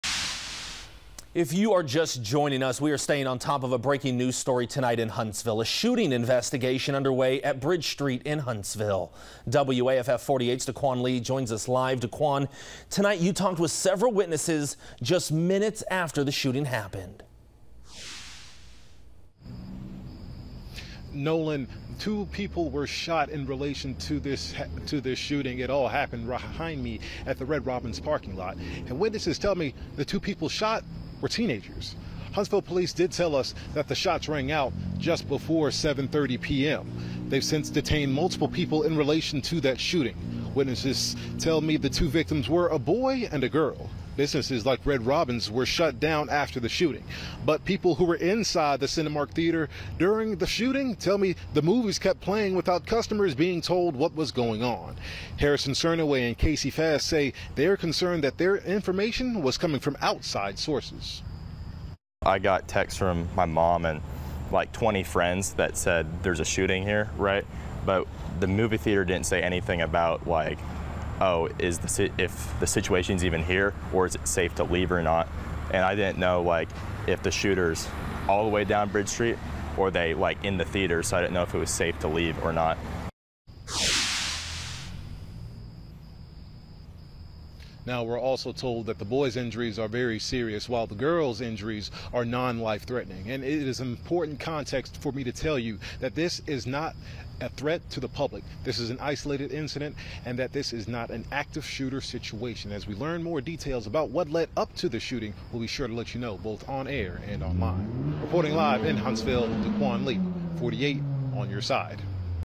Witnesses recount Bridge Street shooting in Huntsville
HEMSI confirms two people were shot, and three people have been detained following the shooting.